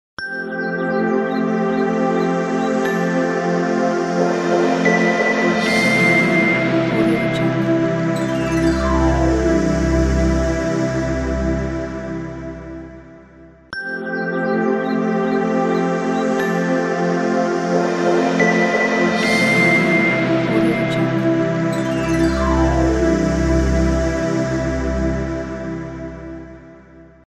Zen Logo | After Effects project | Videohive template
Project features:

Bright and Fun – Awesome Sunny Opener template. You can use it for story creation about travel, holiday, weekend, memories, happiness moments,wedding, love story, baby videos, family album, and also.

This project was developed with 3d studio max , and element 3d ,

the resolution is 1920×1080 but it is easily scalable . included is a video tutorial that will allow you to change it quickly .